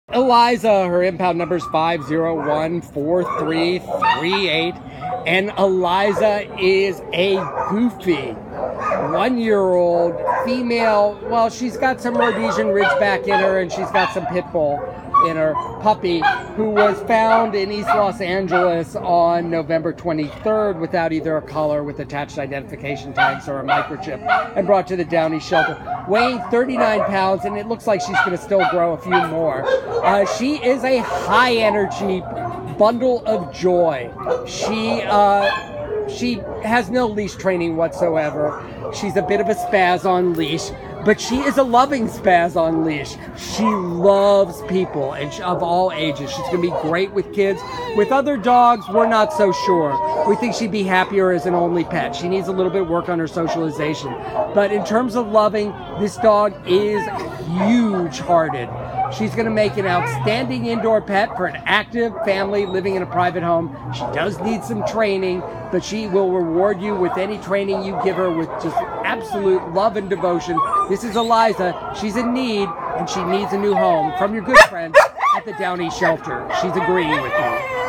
Eliza A5014338
Eliza is an effervescent one year old buff female Rhodesian Ridgeback/Pit Bull mix puppy who was found without either a collar with attached identification tags or a microchip in East Los Angeles on November 23rd and brought to the Downey Shelter. Weighing thirty-nine pounds and likely to grow to forty-five pounds, Eliza has a high energy level and pulls mightily away along on leash.  She has a happy and loving disposition and is quick to dole out kisses.  We think she will be a fabulous pet for children, but she needs to learn how to introduce herself better to other dogs and for right now would do best in a single dog family.   Eliza is a bucket of fun and a tremendous indoor pet for a family living in a private home.